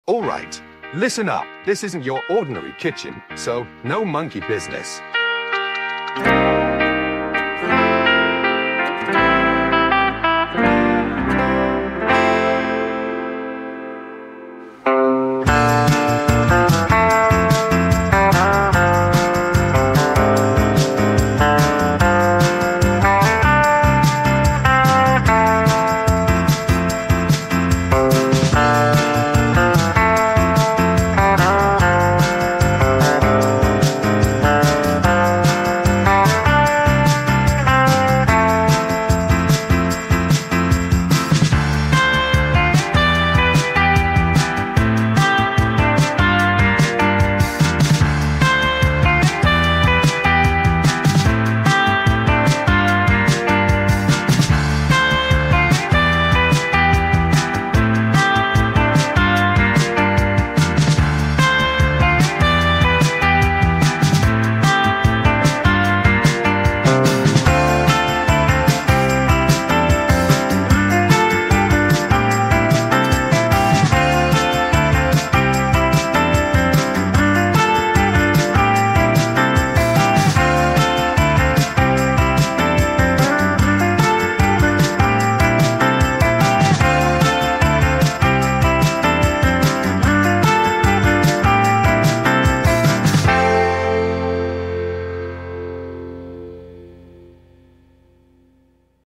You WON'T Believe What AI Gordon Ramsay Just Did in the Kitchen! Funny Ai-Generated Video
Watch as an AI tries to cook like Gordon Ramsay in this hilarious and entertaining video. If you're a fan of Gordon Ramsay or just love funny AI content, you won't want to miss this!